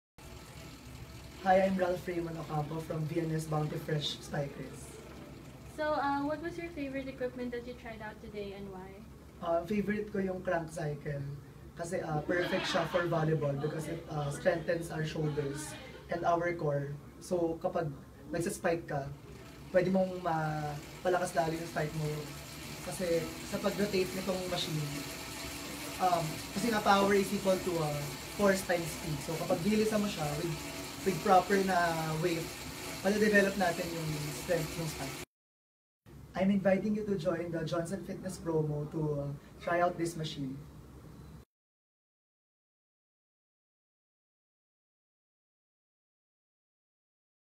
Ralph Ocampo, VNS Bounty Fresh Volleyball Player - Johnson Health Tech Philippines
“Favorite ko ‘yung Krankcycle kase perfect s’ya for volleyball because it strengthens our shoulders and our core. So kapag nagi-spike ka, pwede mong mapalakas lalo ang spike mo.”

– Ralph Ocampo, VNS Bounty Fresh Volleyball Player

Being active makes you smart! \m/

‪#‎fitspiration‬